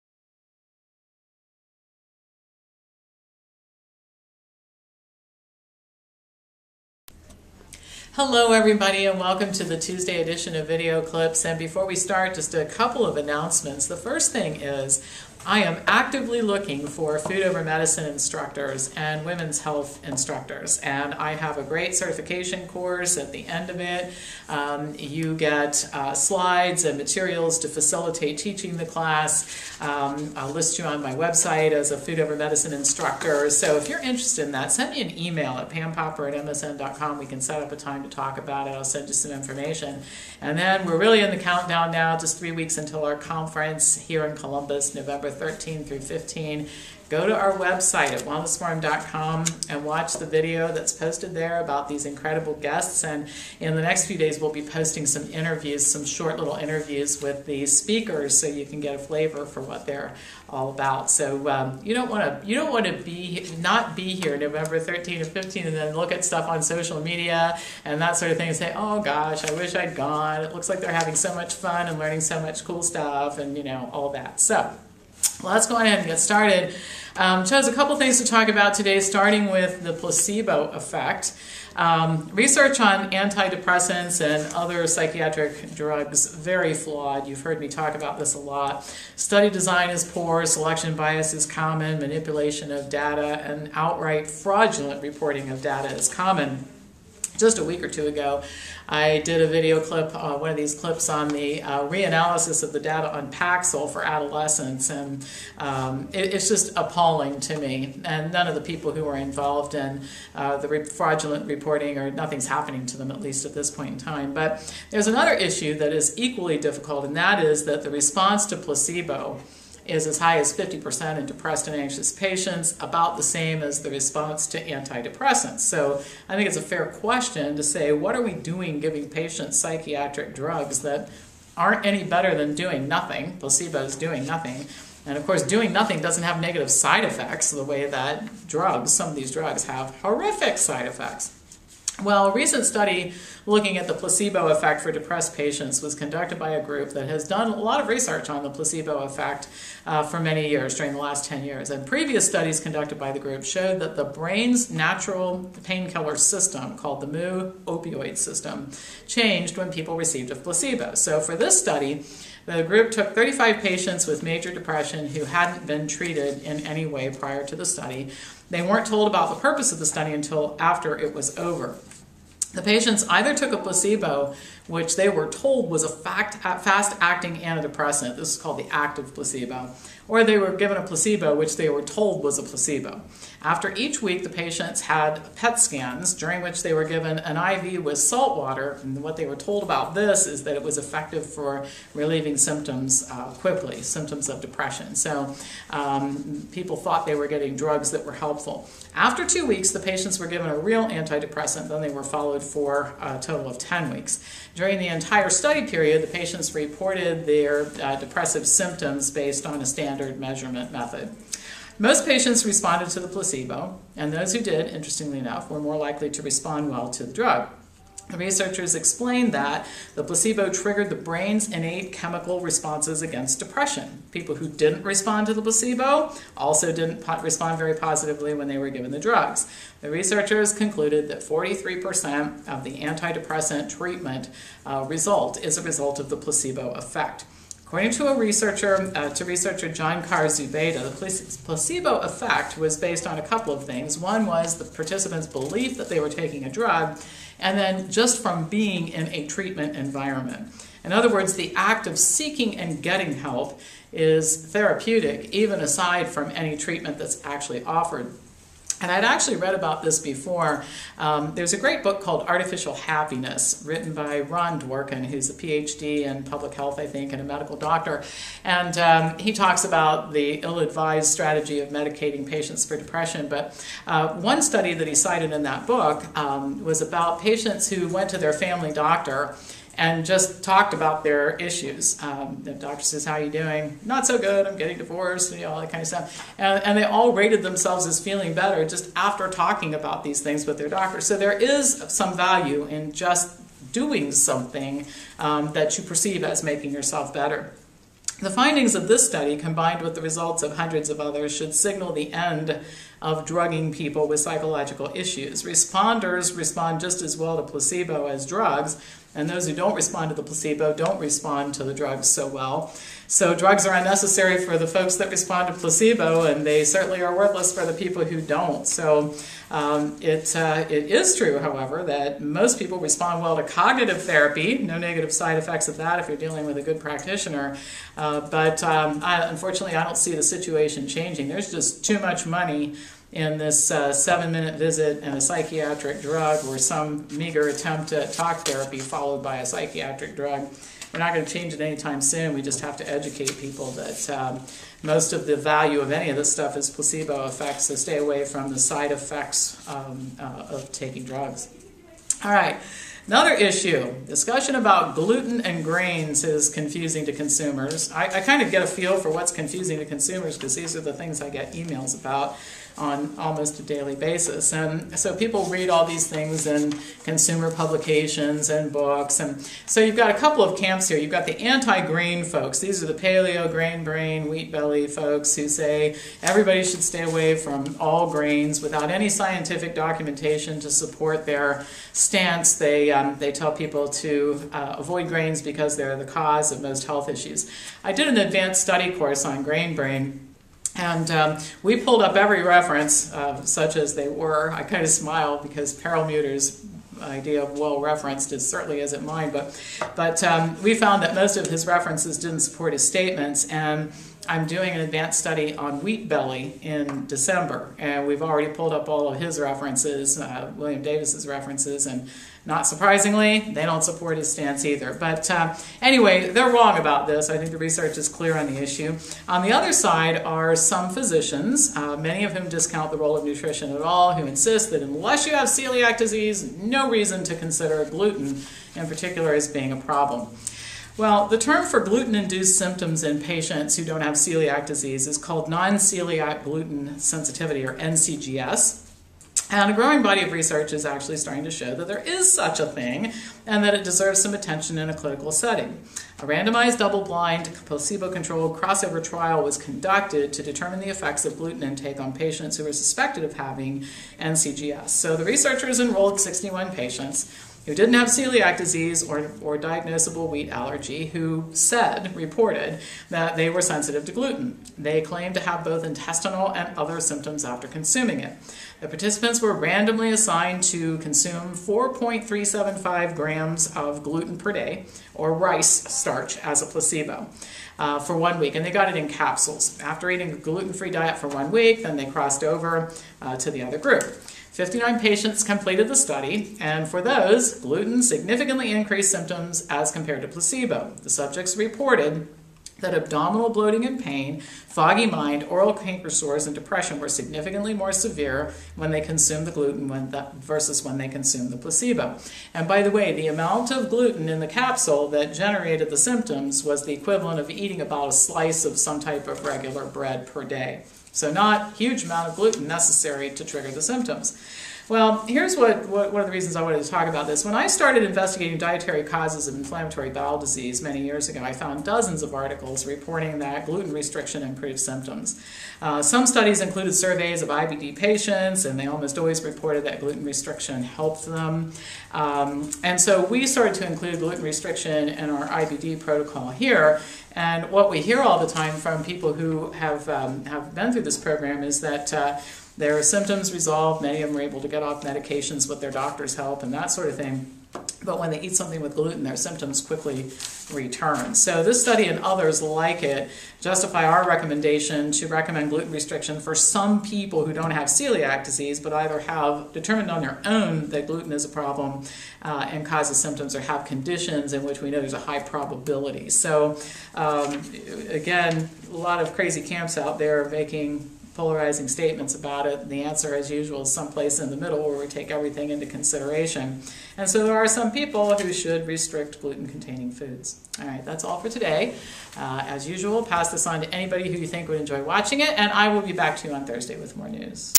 Placebos Are Effective for Relieving Depression AND Non Celiac Gluten Sensitivity
In clinical trials, psychiatric drugs barely do better than placebo, and a new study shows that those who respond to placebo are more likely to respond positively to drugs. It’s time to stop drugging people with psychological issues.

It is true that some people who do not have celiac disease have symptoms related to gluten intake, and benefit from restricting gluten intake.